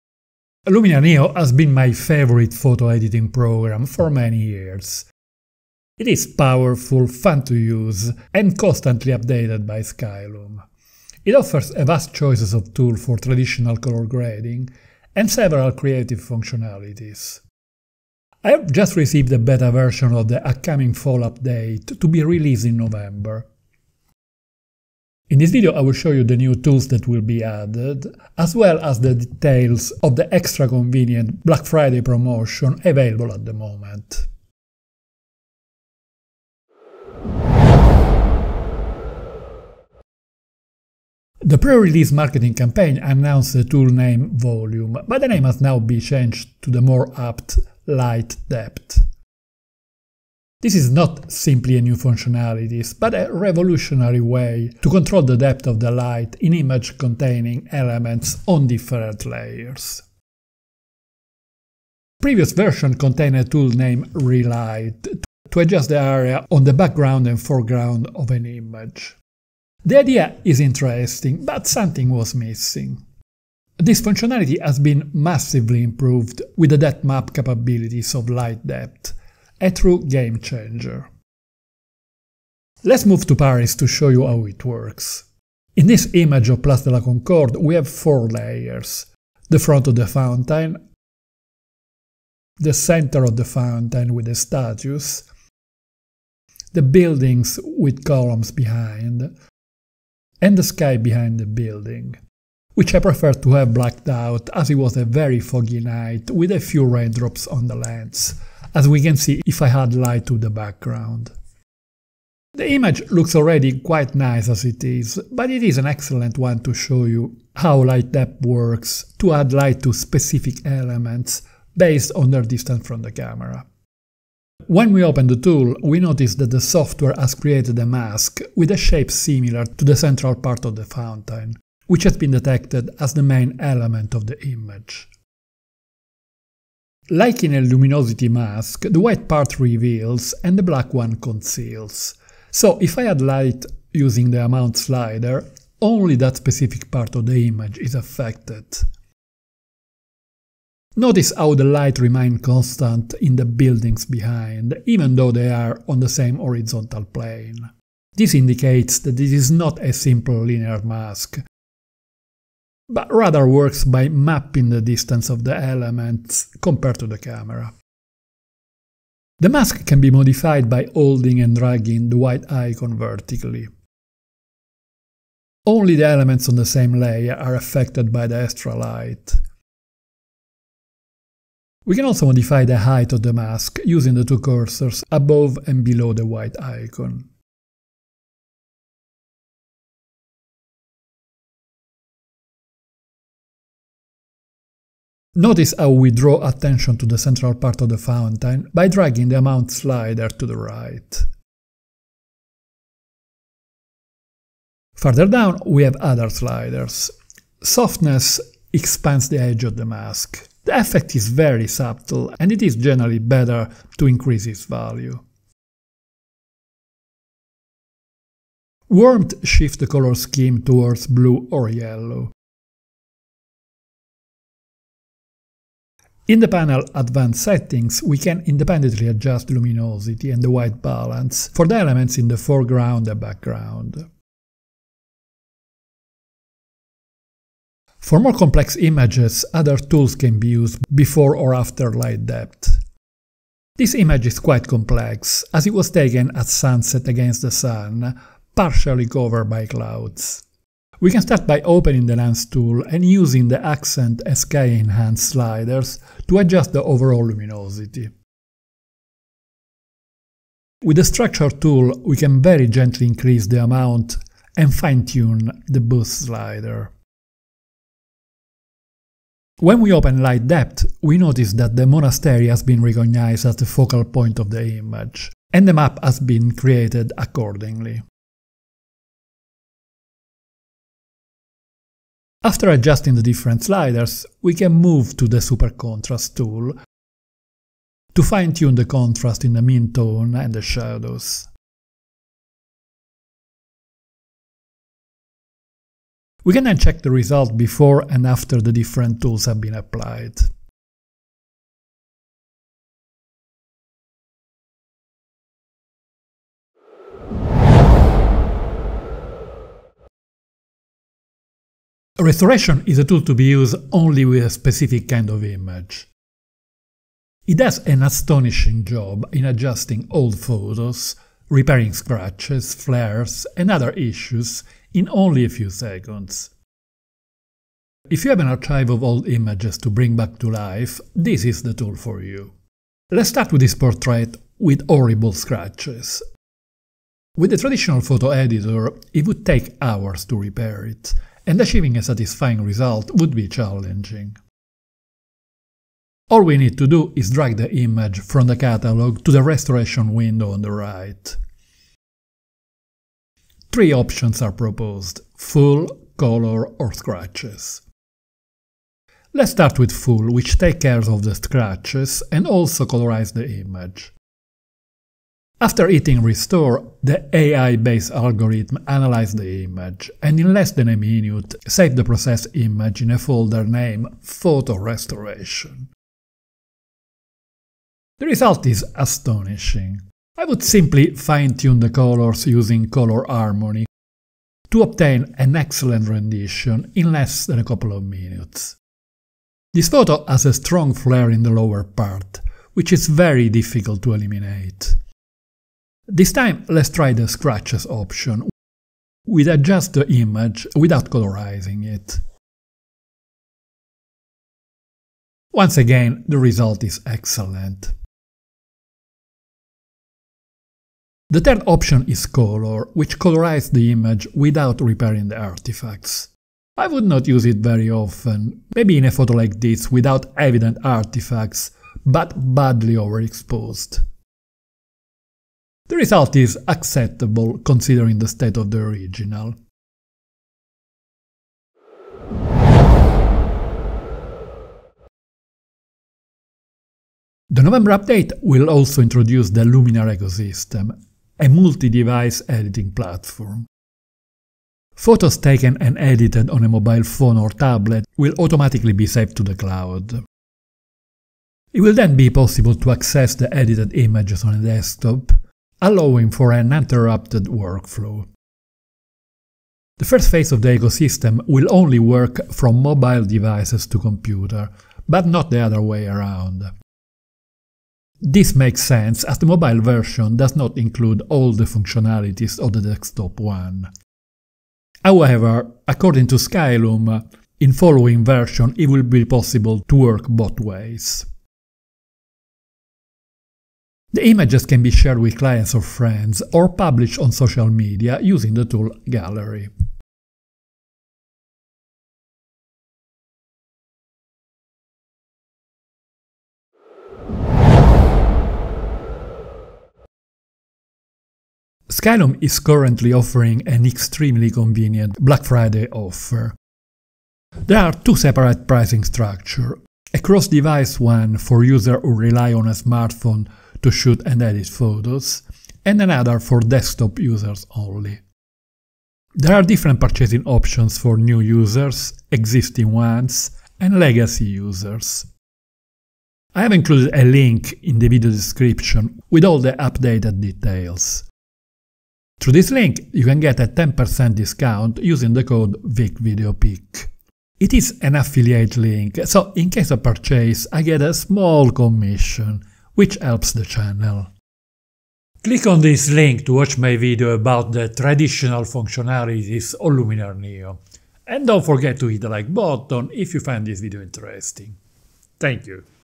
Luminar Neo November 2025 Update - New Tools: LIGHT DEPTH and RESTORATION
Luminar Neo has been my favorite photo editing program for many years. It is powerful, fun to use, and is constantly updated by Skylum. It offers a vast choice of tools for traditional color grading and several creative functionalities. I have received a beta version of the upcoming fall update to be released in November. In this video, I will show you the new tools that will be added, including Light Depth and Restoration, as well as the details of the super-convenient black Friday promotion available at the moment

DJI Flip
DJI Mini 5 Pro
DJI Mini 3
DJI Mini 4k
DJI Neo
DJI Air 3s
DJI Mavic 4 Pro

0:00 Intro
0:37 Light depth
5:07 Restoration
7:12 Ecosystem
8:17 Black Friday Offers